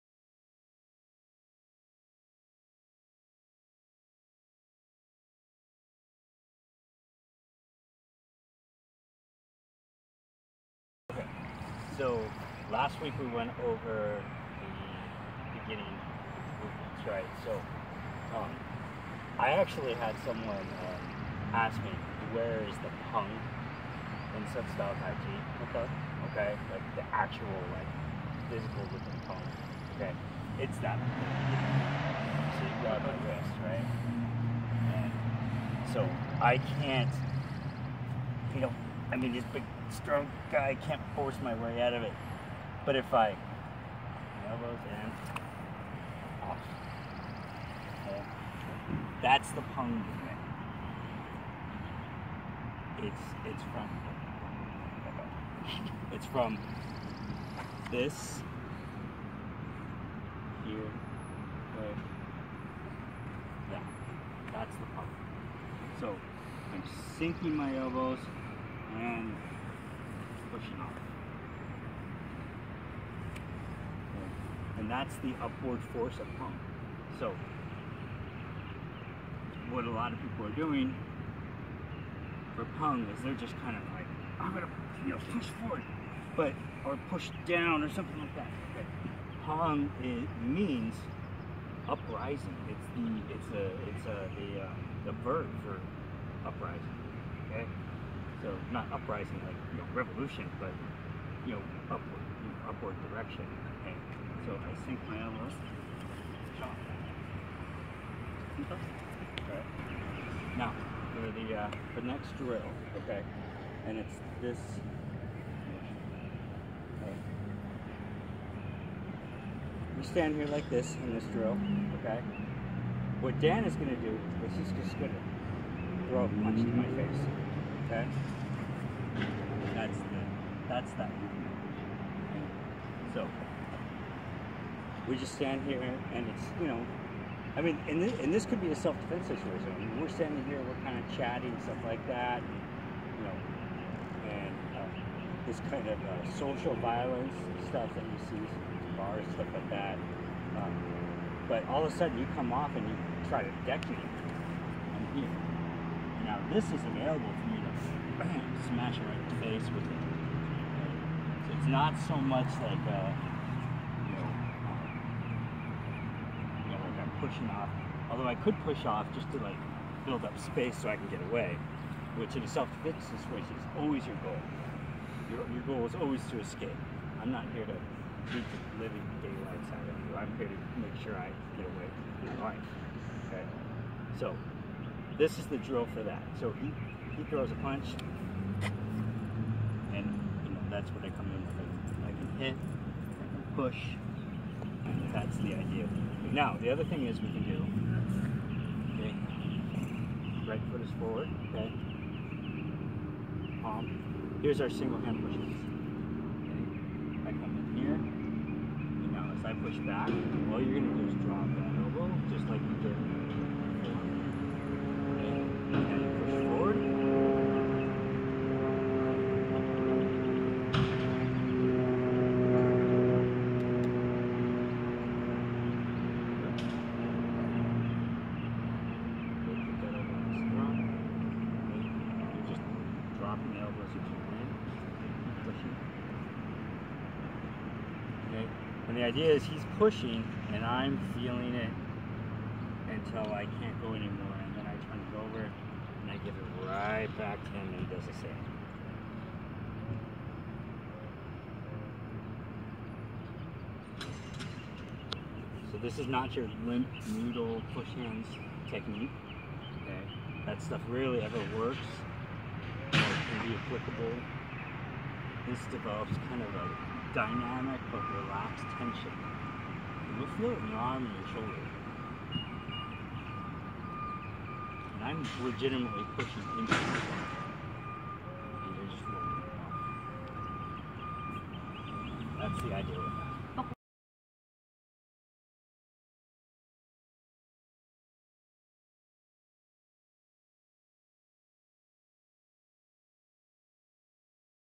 Sun Style Tai Ji Quan's Lazily Tying Up the Clothes (Also Single Push Hands)
The first movements in Sun Style provides foundational drills for taijiquan as martial art and system of defense.  Also included is the single push hand drill to develop the necessary sensitivity for Taijiquan.